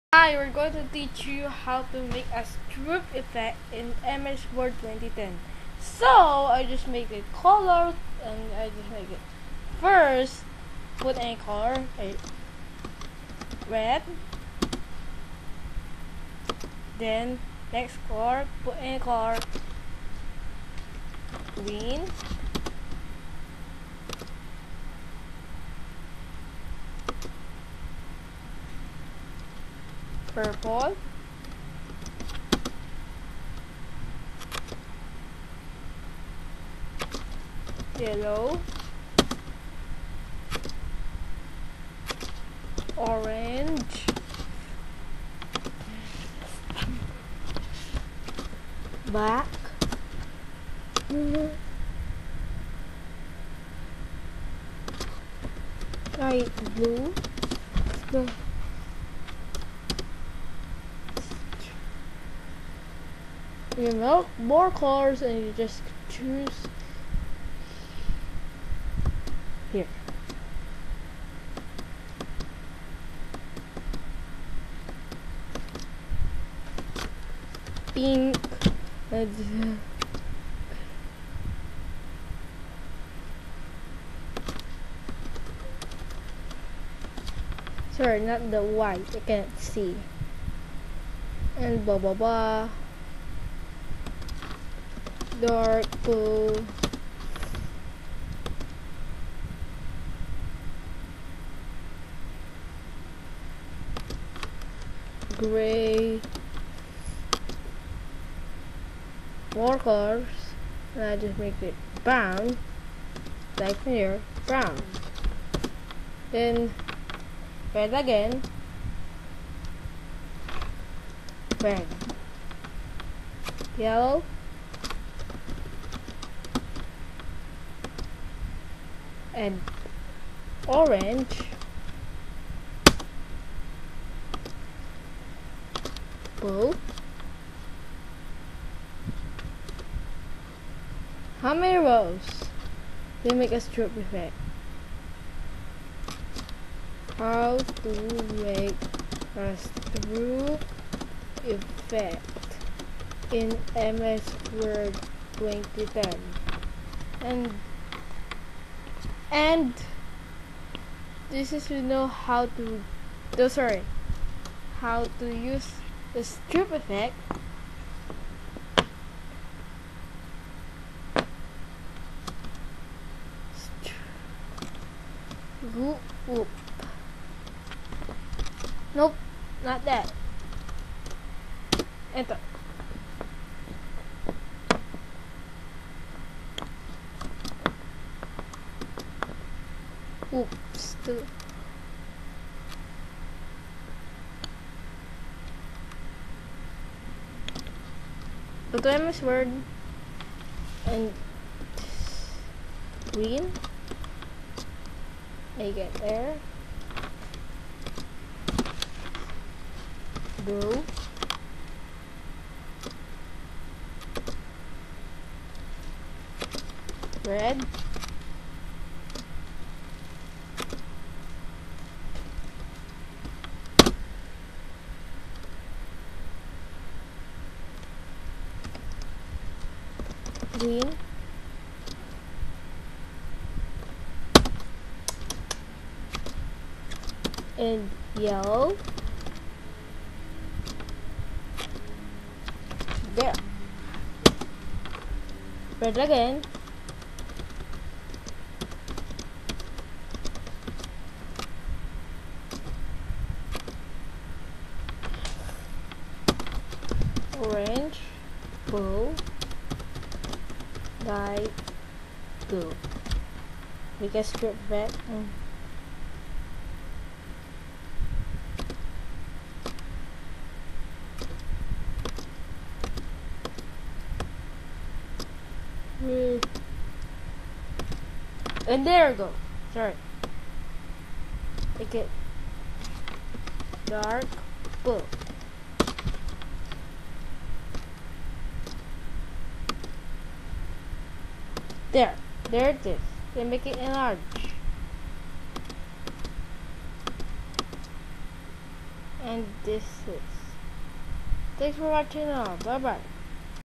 How to make a Stroop Effect in MS Word 2010
This is we know How to make your own Stroop Effect in Microsoft Word 2010